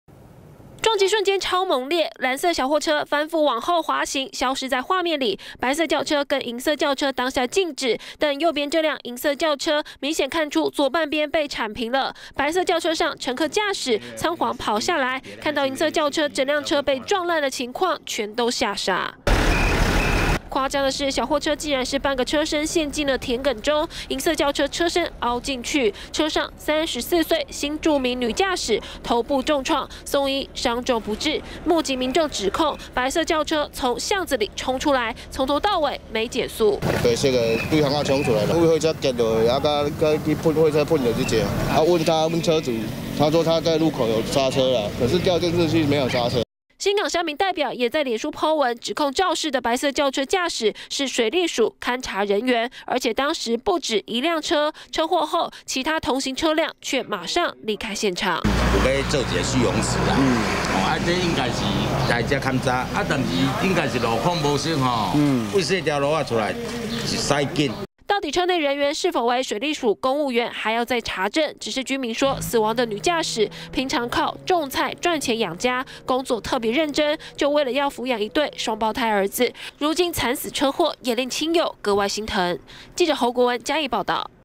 20180308中天新聞 悚！ 3車高速對撞轎車一半遭剷平 女駕駛慘死
嘉義縣新港發生一起重大死亡車禍，一輛轎車撞上小貨車，小貨車追撞另一輛轎車，最後小貨車陷在田埂中，造成轎車女駕駛頭部重創不治。目擊者指控，肇事駕駛一行人，是水利署人員，可能是南下勘查工程進度，卻因為路況不熟釀禍！